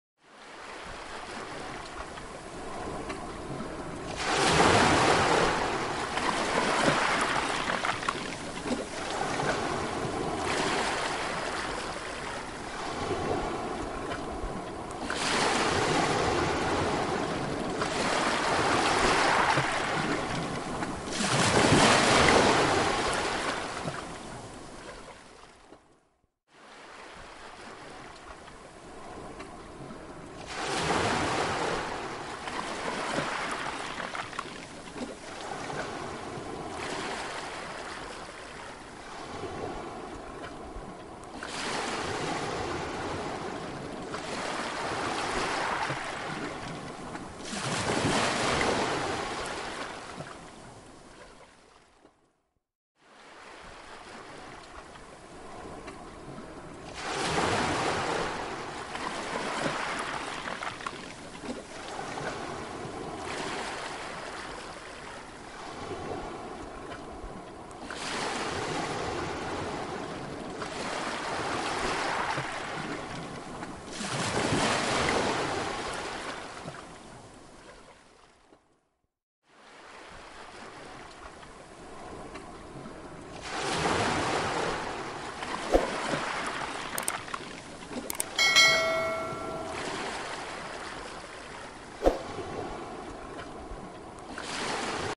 sonido de olas sanadoras del mar
Desde tiempos milenarios, se le atribuyen poderes, relajantes y curativos al sonido de las olas del mar, muchos las llaman sanadoras, haz la prueba